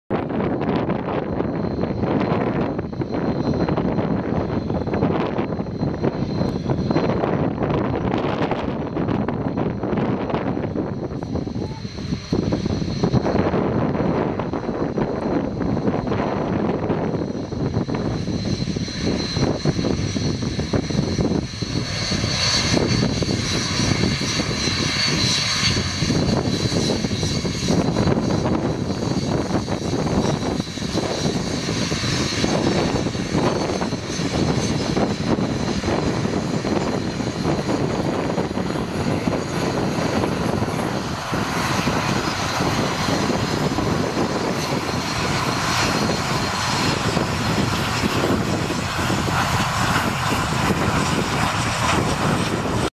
Red Arrows taxing at RAF Valley
The Red Arrows taxing to start their display on the RAF Valley families day 28th May 2015